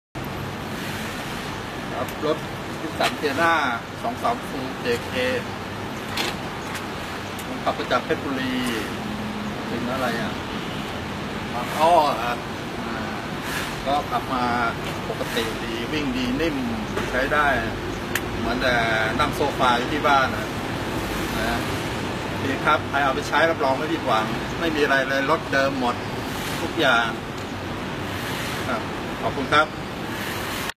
เสียงแห่งความประทับใจของลูกค้ารถมือสอง Admire #6
เพราะอะไรหนอทำให้คุณคึกฤทธิ์ขับรถสุดรักมาจากเพชรบุรีเพื่อส่งต่อให้บางอ้อได้ดูแลต่อไป
ขอบคุณคุณคึกฤทธิ์ที่ได้กรุณาพูดถึงรถคันนี้และขอบคุณที่ท่านวางใจขายรถให้กับบางอ้อค่ะ.. NISSAN TEANA 230JM ขับมาจากเพชรบุรีถึงบางอ้อ ก็ขับมาปกติดีนิ่ม ใช้ได้เหมือนนั่งโซฟาที่บ้าน... โอเคครับใครเอาไปใช้ต่อ ไม่ผิดหวัง ไม่มีอะไรเลยรถเดิมหมดทุกอย่าง.

โชว์รูมรถยนต์มือสองเหนือมาตรฐาน โปร่งใส ใส่ใจคุณภาพ ดำเนินงานมากว่า 47 ปี